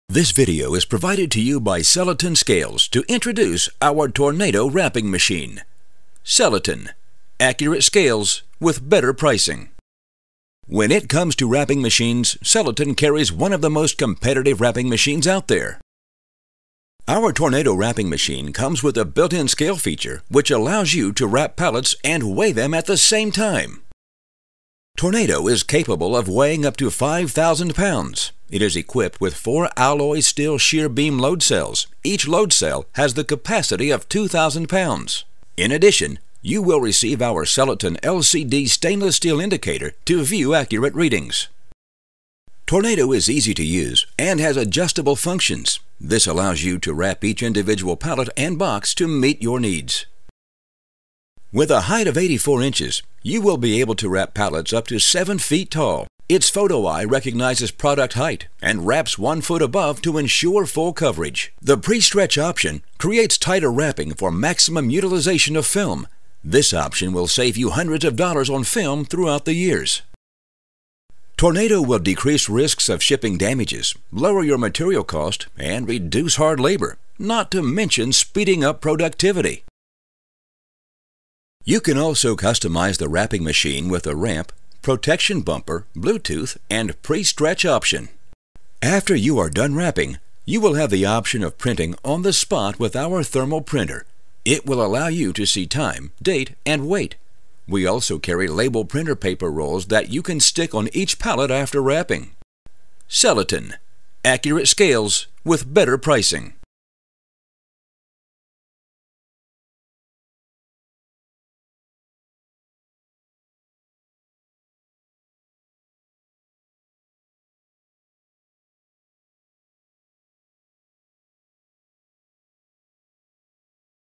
SellEton Smart Stretch Wrapping machine with built-in Scale
When it comes to wrapping machines, SellEton Carries one of the most competitive wrapping machines out there!

Our “Tornado” Wrapping machine comes with a built-in scale feature, which it allows you to wrap pallets & weigh them at the same time!

Tornado is capable of weighing up to 5,000 lbs. it is equipped with 4 alloy steel shear beam load cells, each load cell has the capacity of

2000 pounds. In addition, you will receive our SellEton LCD stainless steal indicator to view accurate readings.

Tornado is easy to use & has adjustable functions. this allows you to wrap each individual pallet & box to meet your needs.

With height of 84” , you will be able to wrap pallets up to 7 feet tall. It’s photo eye recognizes product height & wraps 1 foot above to insure

full coverage. The pre-stretch option creates tighter wrapping for maximum utilization of the film. This option will save you hundreds of

dollars on film through out the years.

Tornado will decrease risks of shipping damages, lower your material cost & reduce hard labor. Not to mention speeding up productivity!

You can also customize the wrapping machine with a ramp, protection bumper, bluetooth & pre-stretch option.

After your are done wrapping, you have the option of printing on the spot with our thermal printer. it will allow you to see time, date & weight.

 We also carry label printer paper rolls so you can stick on each pallet after wrapping.